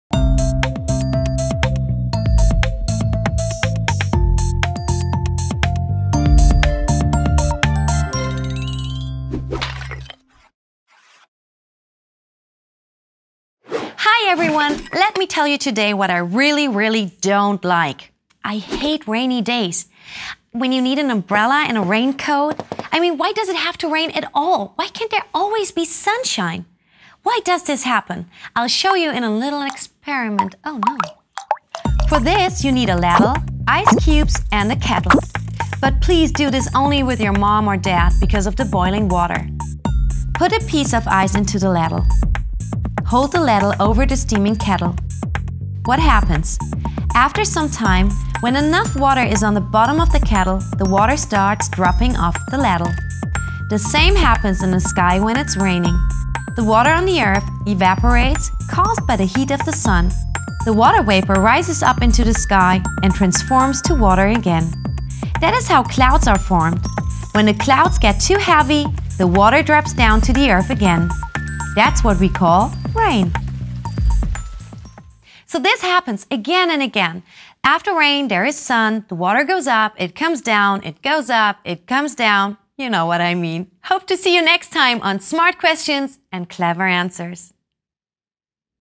Why does it rain? Smart question, clever answer!
Cold and rainy days - everybody hates that! But why does it rain? Nina got the right answer for this smart question!